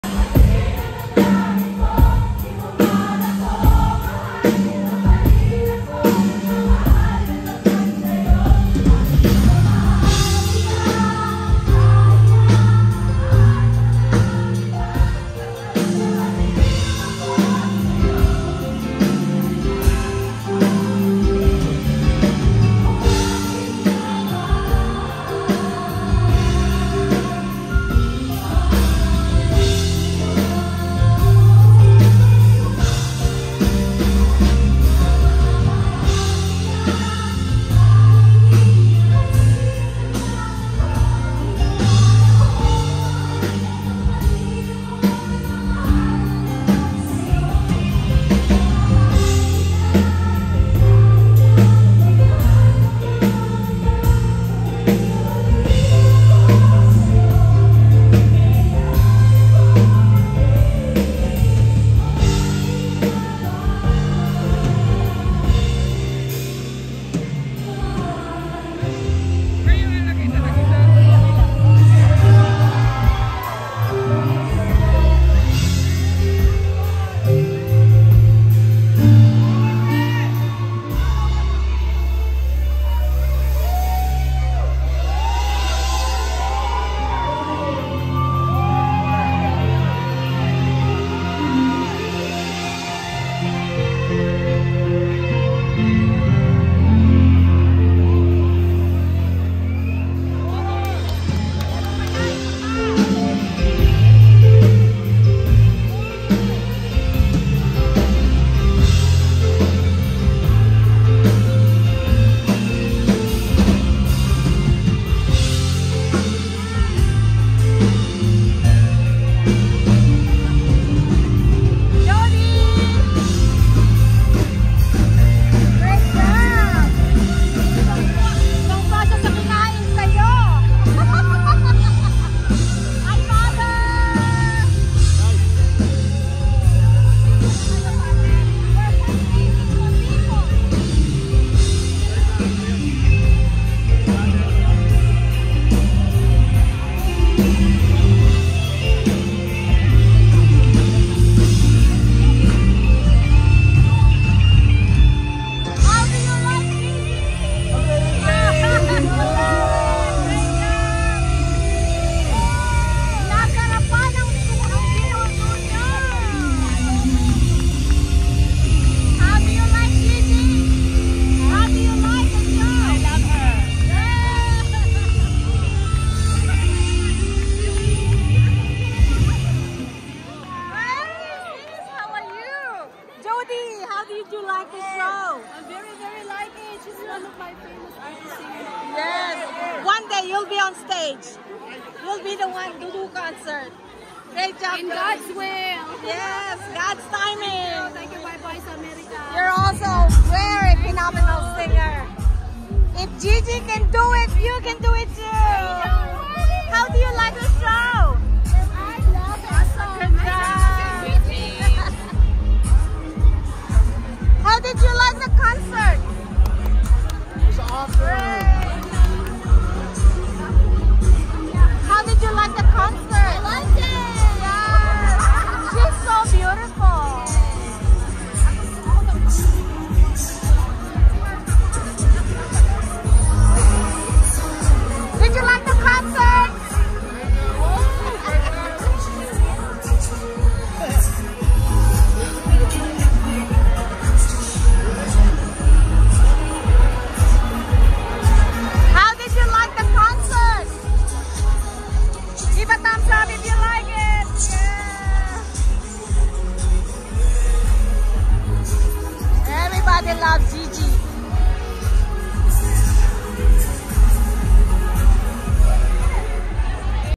Gigi De Lana's final song in Arizona and People's Reaction to her concert
Gigi De Lana's final song in Arizona and People's Reaction to her concert gigivibes produced by M & A Gold Productions